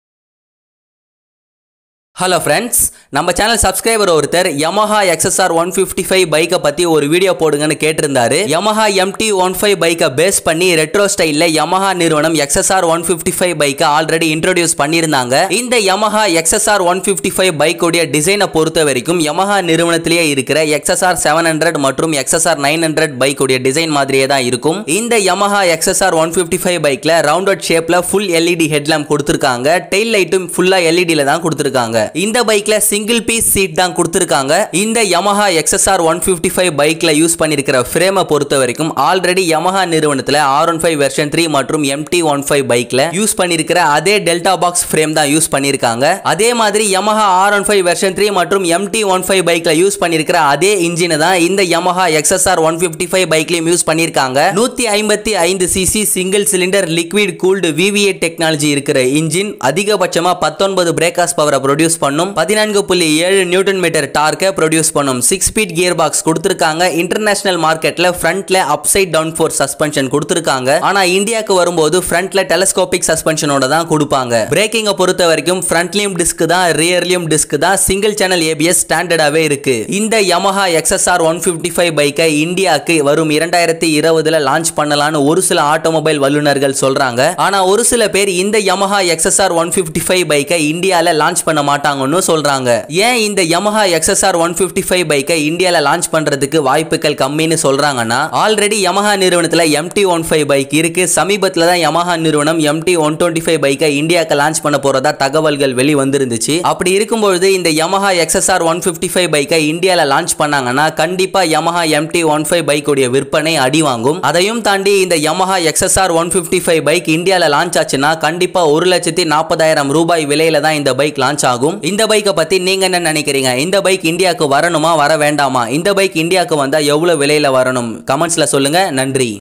Will Yamaha launch Yamaha xsr 155 bike in India ? | தமிழில் | Mech Tamil Nahom
YAMAHA company already introduced yamaha xsr 155 in Thailand but we don't know will yamaha launch xsr 155 bike india so in this video i will clear it and this video will be explained in Tamil language and thank you for watching this video